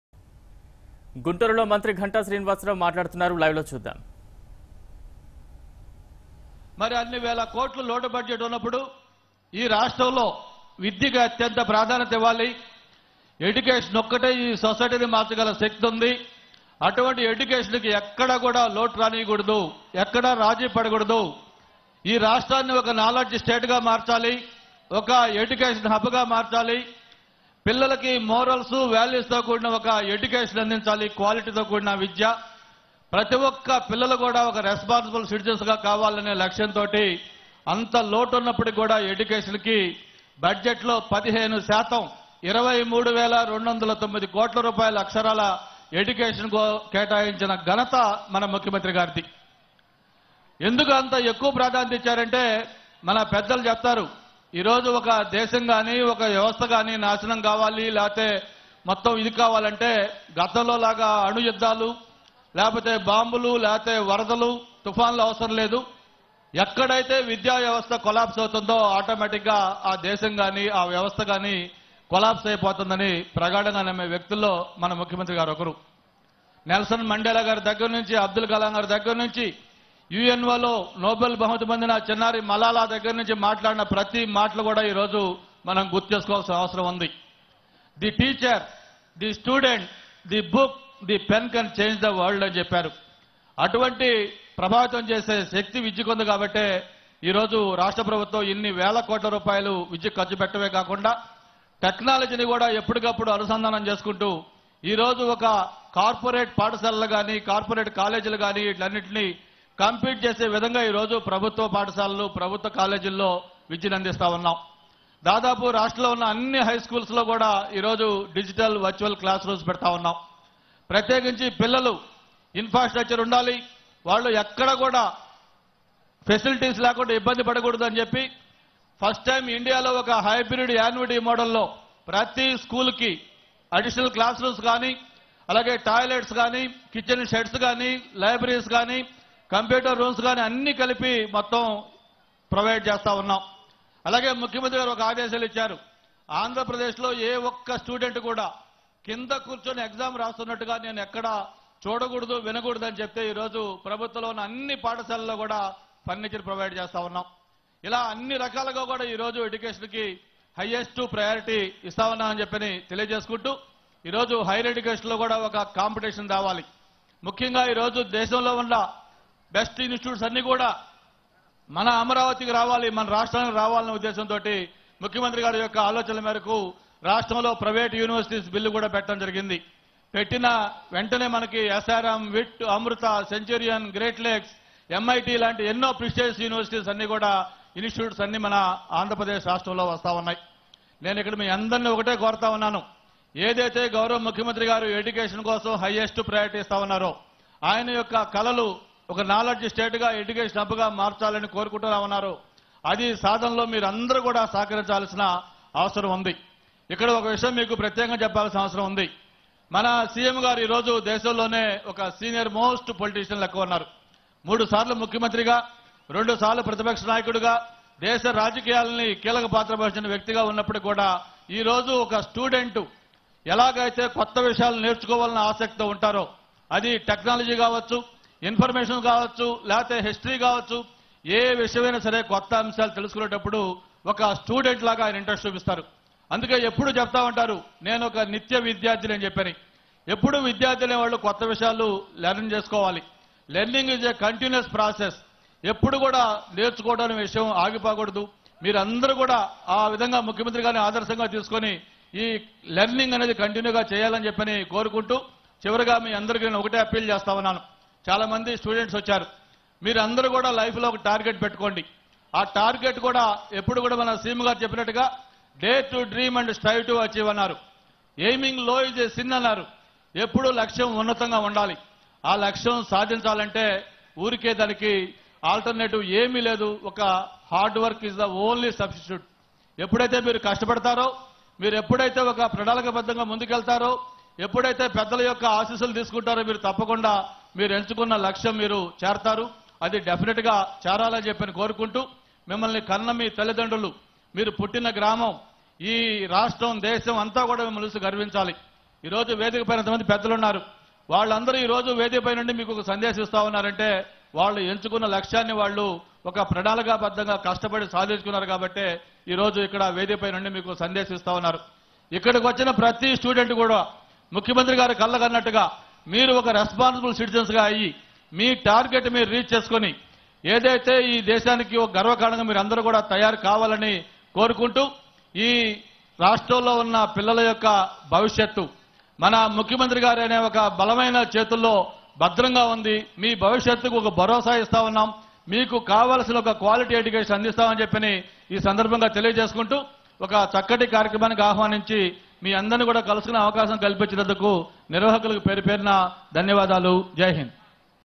Minister Ganta Srinivasa Rao Speech At Kotappakonda | University College Of Engineering
Minister Ganta Srinivasa Rao Speech At Kotappakonda | University College Of Engineering | Bharat Today